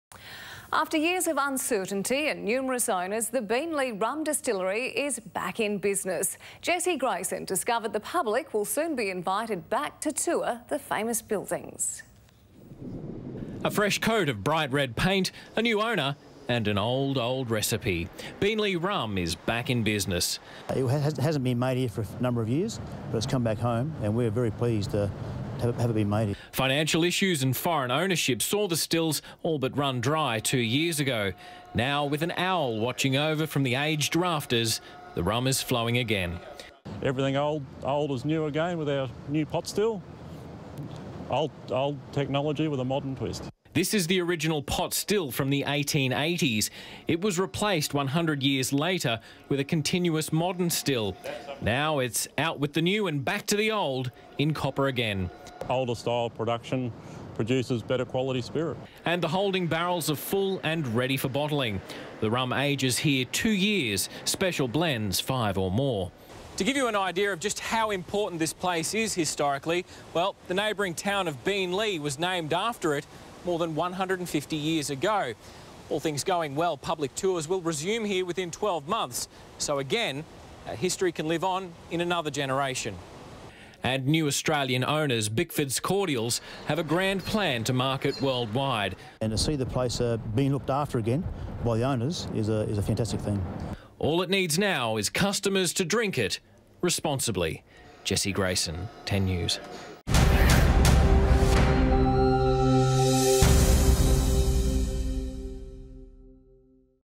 Back in Business
After years of uncertainty and numerous owners, the Beenleigh Rum Distillery is back in business.